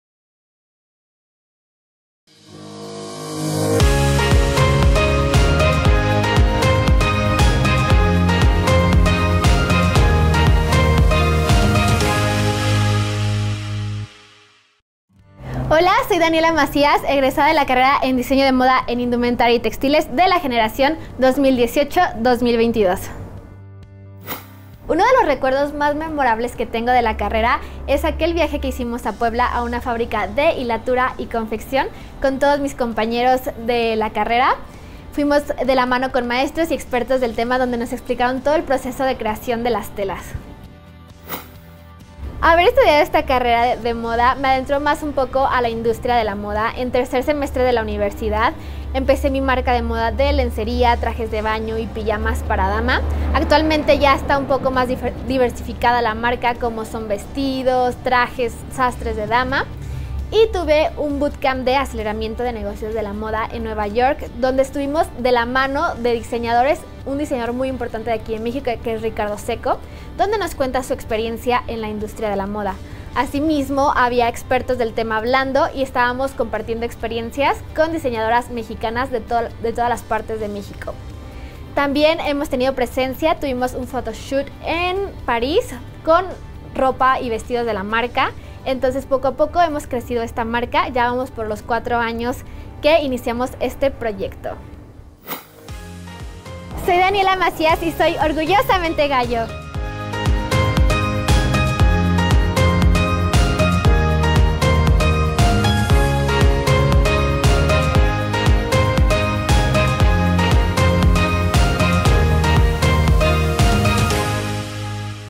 50 Historias de Luz - Daniela Macías
Conoce la historia de Daniela Macías egresada de la UAA de la carrera de Diseño de Moda en Indumentaria y Textiles dedicada a su empresa Danielle en Aguascalientes.